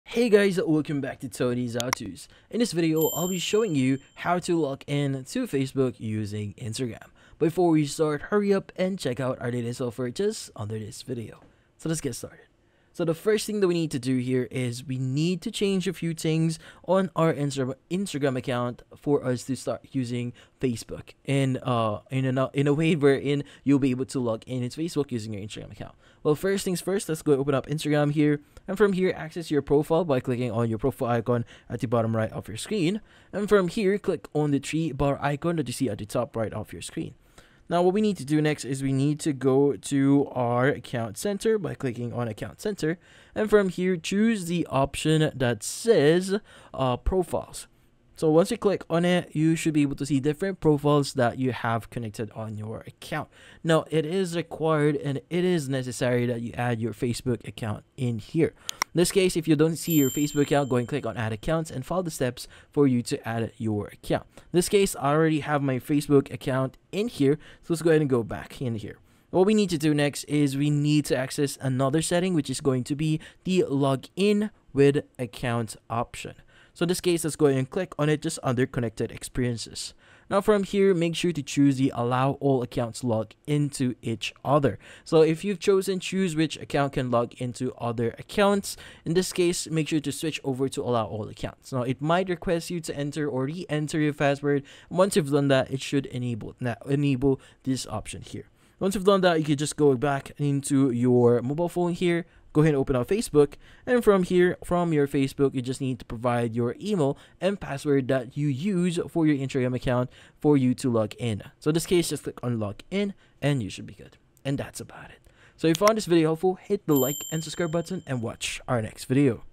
How to Login to Facebook Using Instagram (2026)
In this video I show you How to Login to Facebook Using Instagram in 2026. The quick and easy tutorial how to sign into Facebook with insta. Do you want to know step by step how to signin facebook account with instagram in 2026? Yes? Great!